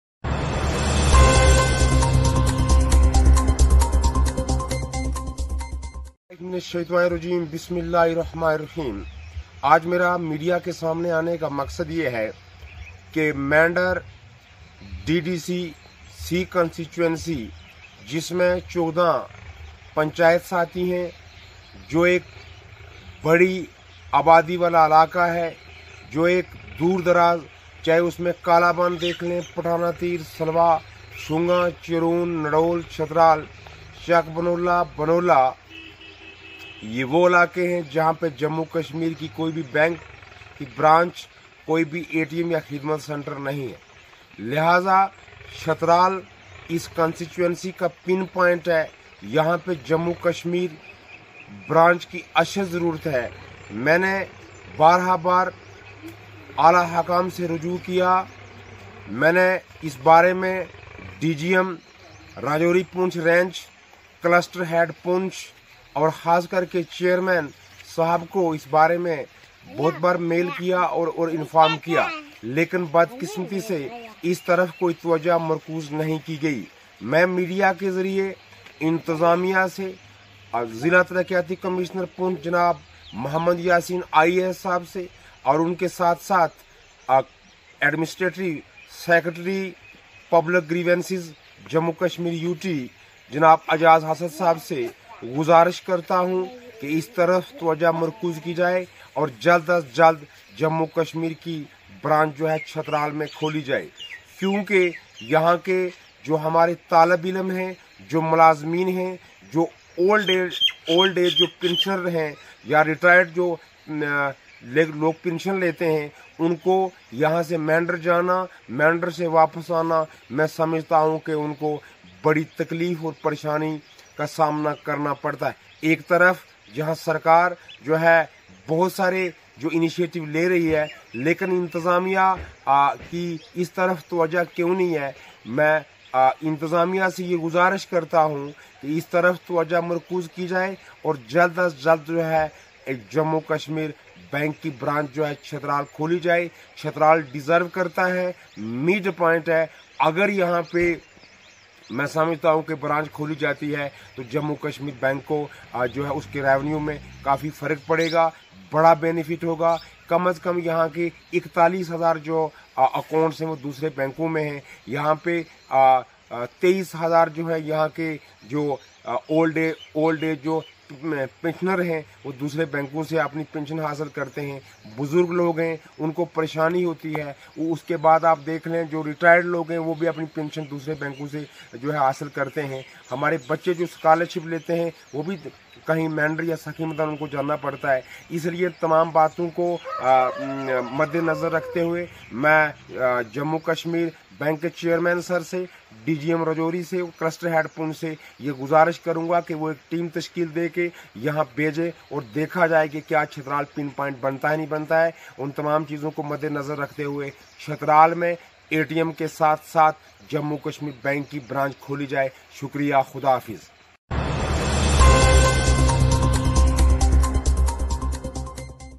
Branch of Jammu and Kashmir Bank should be opened in Nabiet Chhatral of Mendhar SubDivision
A Branch of Jammu and Kashmir Bank should be opened in Nabiet Chhatral of Mendhar SubDivision as soon as possible. People are facing various problems. Tanveer Iqbal Qureshi

Mendhar//Tanveer Iqbal Qureshi Spokesperson Bhartiya Janata Party Pahari Culture and Welfare Cell JK UTSaid  that Chhatral Nabiet of Mendhar SubDivision of Poonch Distt. is the pivot of 14 panchayats with ample road connectivity.Moreover Higher secondary school Chhatral, Primary health centre Chhatral, Power station, Post office and Niabat Chhatral also lies in its vicinity.I would like to mention here that this District Development Council (Mendhar C)compromises of about 50 thousand population with 88 government schools,14895 students,18745 old age pensioners,8230 retired pensioners, total accounts of  different banks 42294.